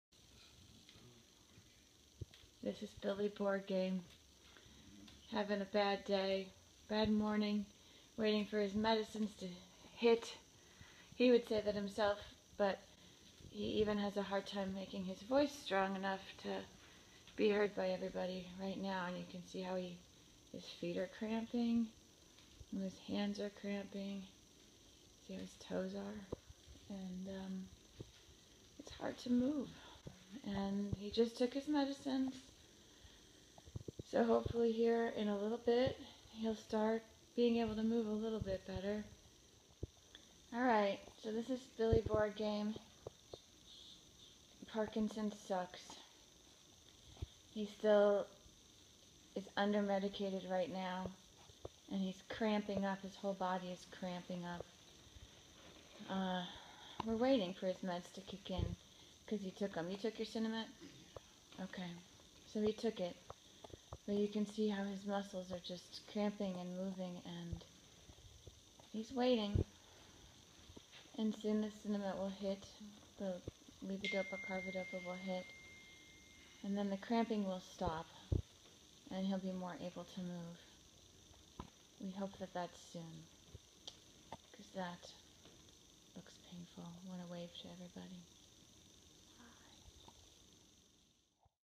Billy Board Game - Time Off
Here we have the Parkinson's patient in the morning. It's a crap shoot whether I wake up like this or not. Usually if I don't sleep well I can look forward to something like this.